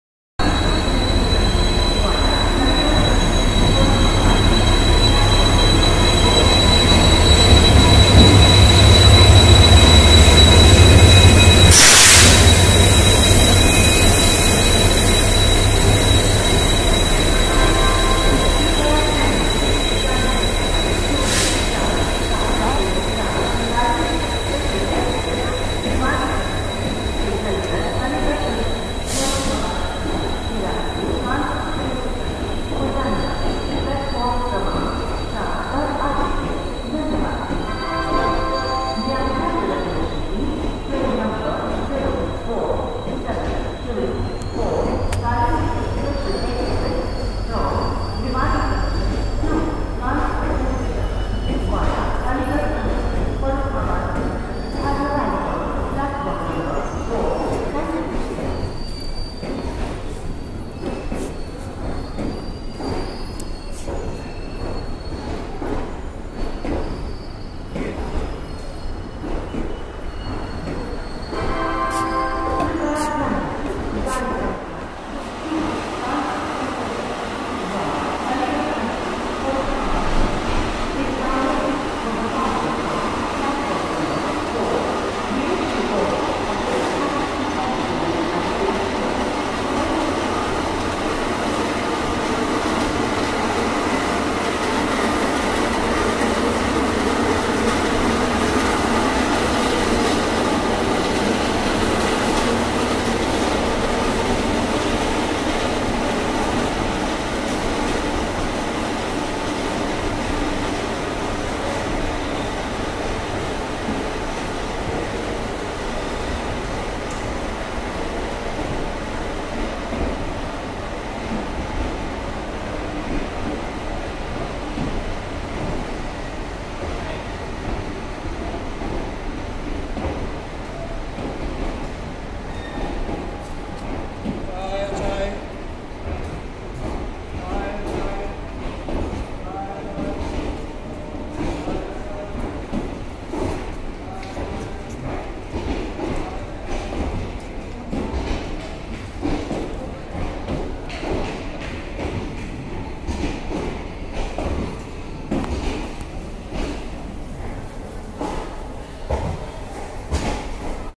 Kalindi Express Arrival and Departure at Ghaziabad jn | 04724 Bhiwani Kanpur Central Via Farukhabad
Train no. 04724 Bhiwani Kanpur Central Kalindi Express with BGKT WDP4B Locomotive arriving and Departing Ghaziabad jn Pf 4.